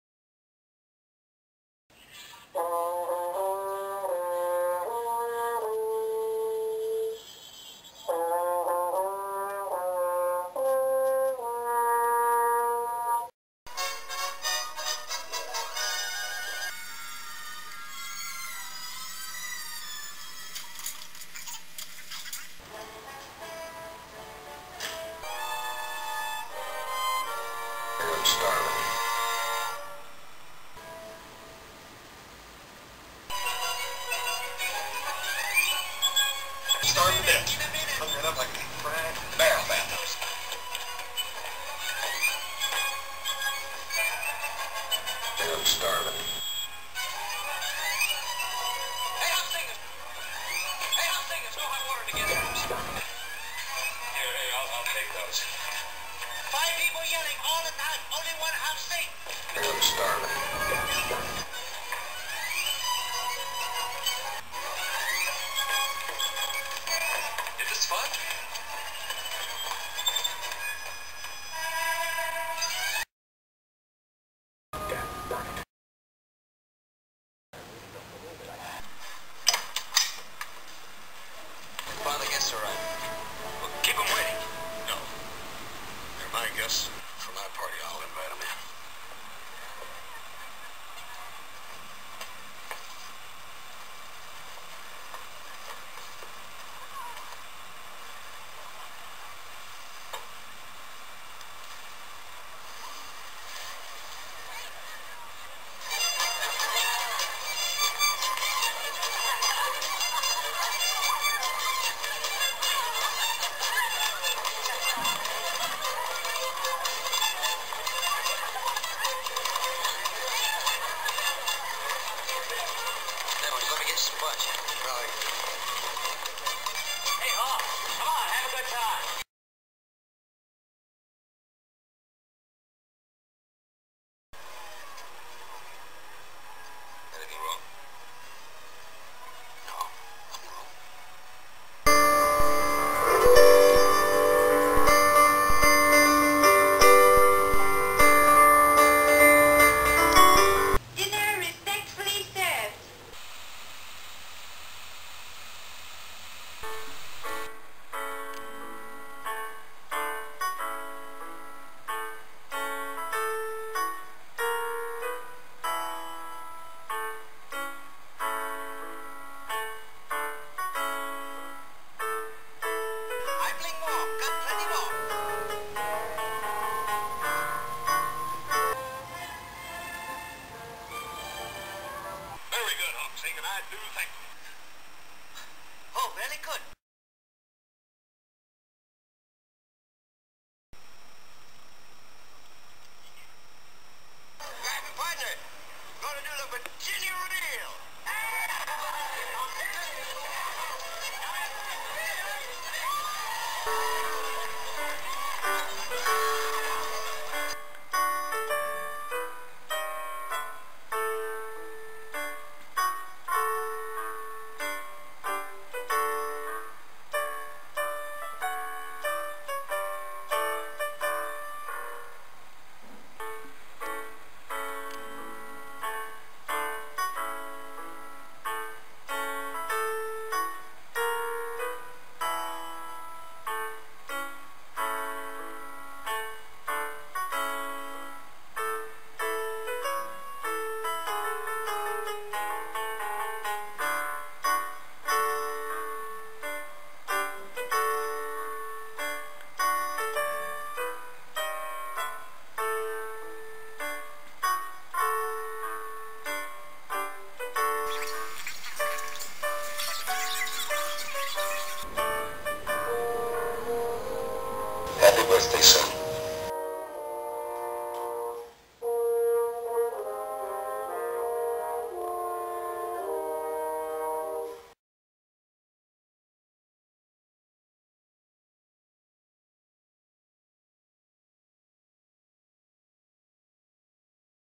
Late(st) Party for Hoss - Birthday 2012
Sorry i am a bit late with this tribute for Dan Blocker's Birthday on 10/12/2012 - there was a problem with our net connection, which didn't allow me to upload or watch anything for almost 1 week!:(
Matching circumstances... please forgive me, Hoss!